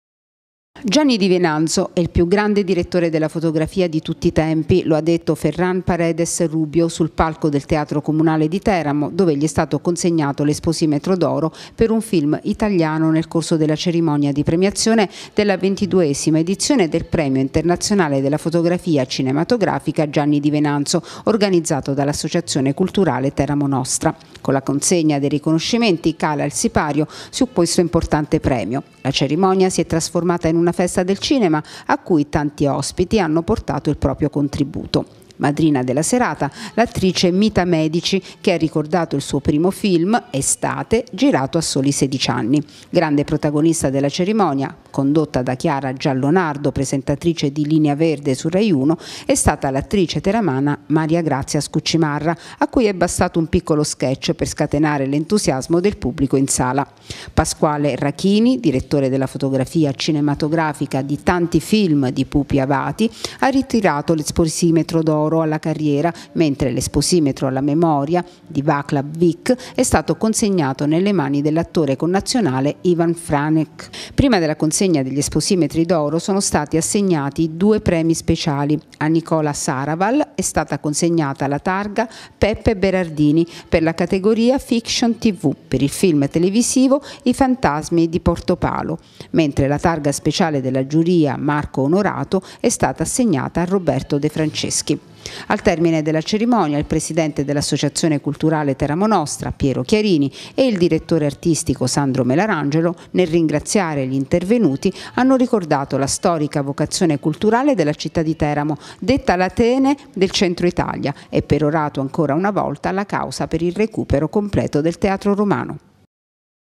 22° edizione del Premio Di Venanzo
Con la consegna degli esposimetri d’oro, sabato scorso è calato il sipario sulla 22° edizione del Premio internazionale della fotografia cinematografica Gianni Di Venanzo, che, come ogni anno, si tiene a Teramo e ricorda la figura di uno dei maestri della luce nel cinema. La cerimonia è stata una vera e propria festa del cinema